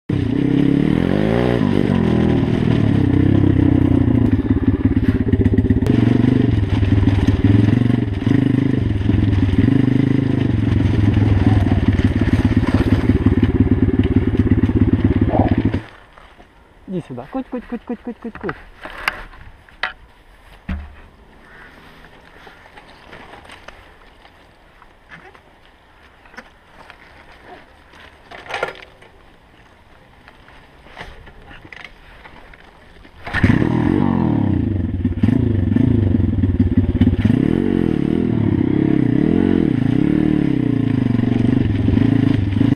Viral Video UK: Biker finds abandoned dog tied up in sack
Denis Polyakov was biking through some dormant train tracks when he discovered an abandoned dog tied up in a sack. Clearly this was deliberate and Denis notified the authorities. So far no one has been prosecuted.